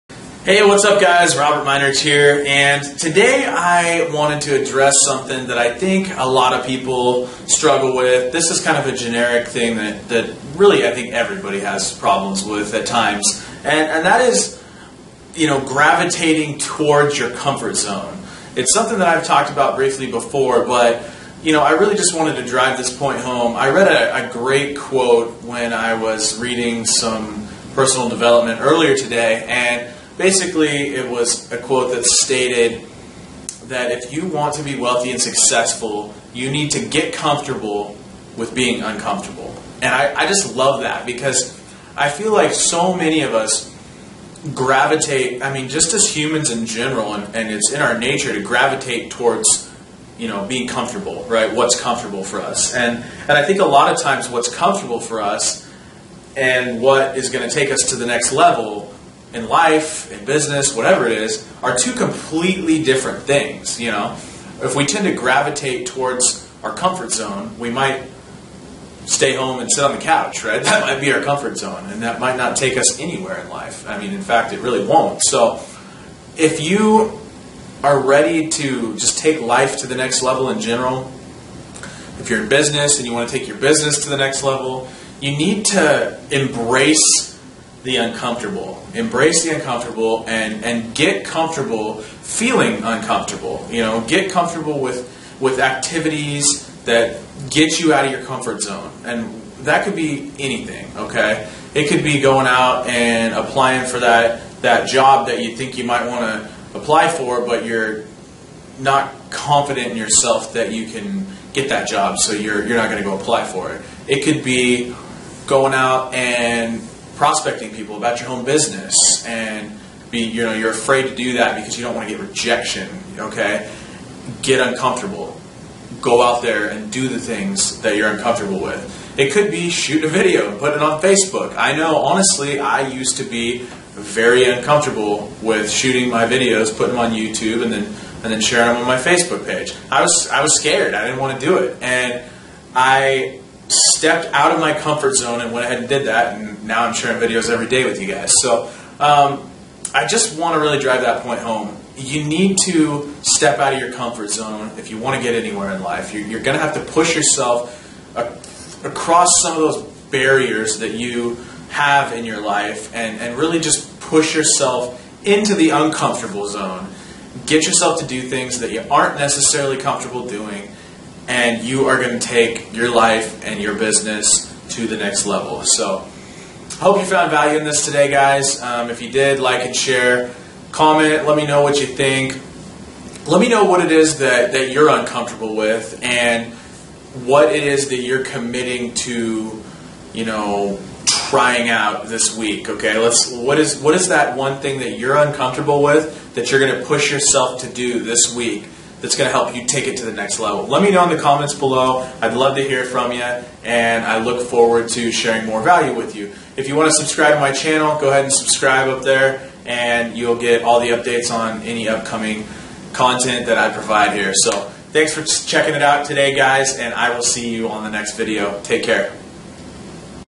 Are You Comfortable With Getting Uncomfortable?
Are you shying away from uncomfortable activities? This is hurting your business and your success!